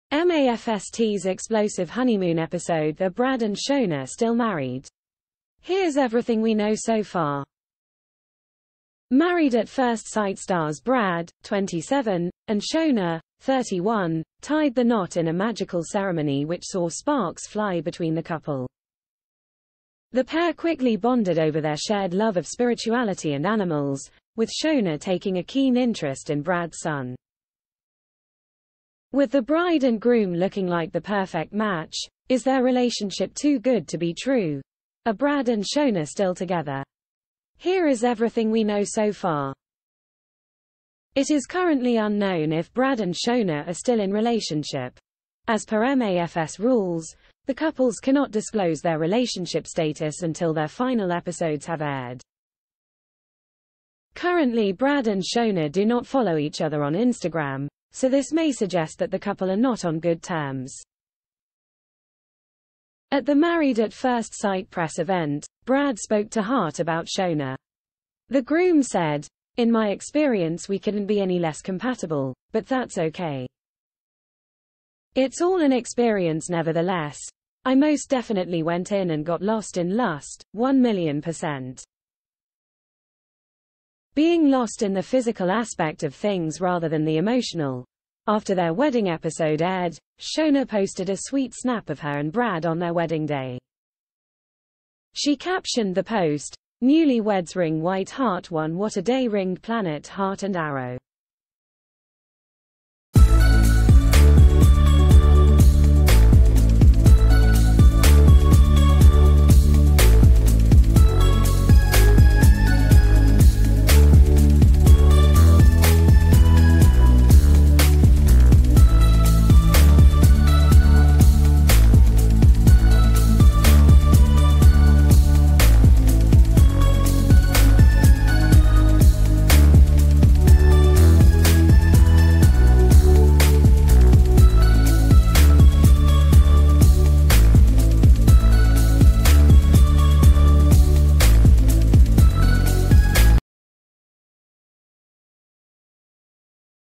Married At First Sight: Are Brad and Shona still together?
MAFS tease explosive honeymoon episode   Are Brad and Shona still married? Here's everything we know so far.  Married At First Sight stars Brad, 27, and Shona, 31, tied the knot in a magical ceremony which saw sparks fly between the couple.The pair quickly bonded over their shared love of spirituality and animals, with Shona taking a keen interest in Brad's son.With the bride and groom looking like the perfect match, is their relationship too good to be true?Are Brad and Shona still together? H...